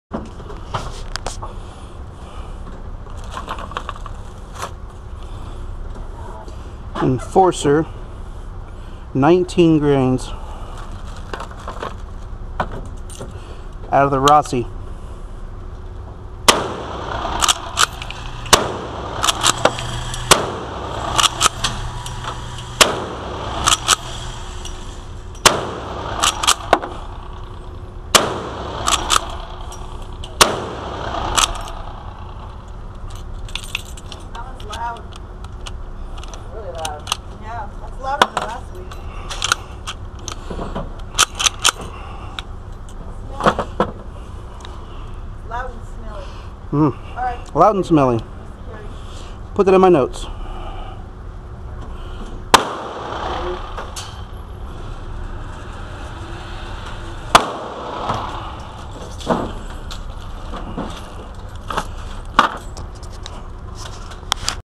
Loud & Smelly 19gr Enforcer out RRH 60fps 44 mag
Remington 44 magnum
19.0gr Ramshot Enforcer
1.600 COAL
Mixed Brass
Tula LPP
240gr Plated RN (Xtreme Bullets)
Loud and Smelly magnum load shot out of Rossi Ranch Hand in .44 MAG